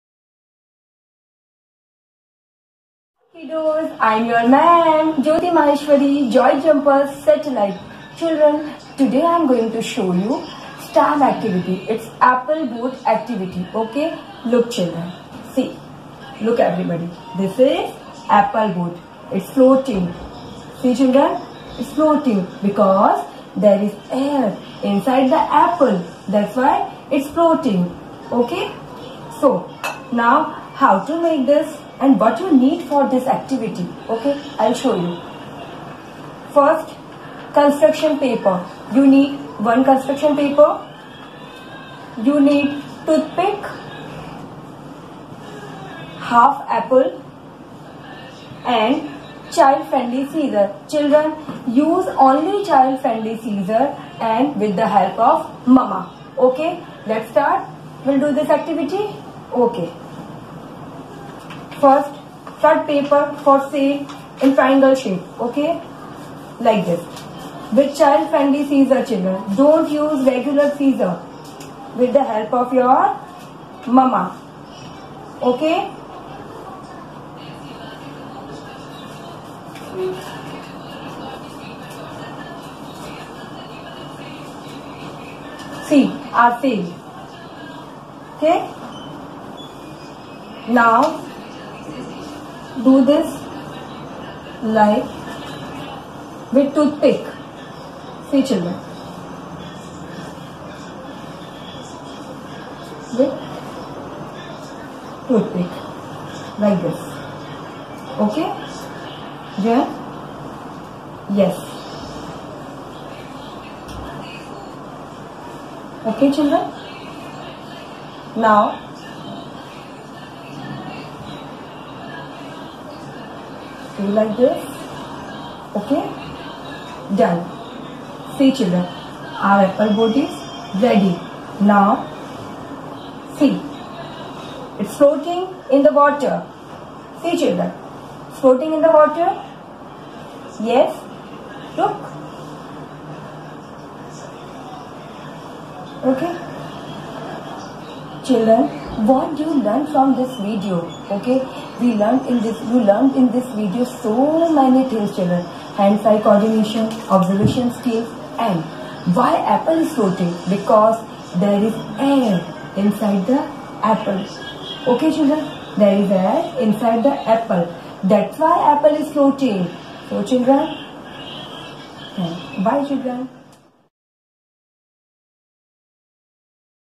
JJ Satellite I STEM Apple Boat
STEM is important because it pervades every part of our lives. STEM activities gives hands-on and minds-on lessons to the children.
Here is an Educator from JoyJumperz Satellite is presenting how to create an Apple Boat !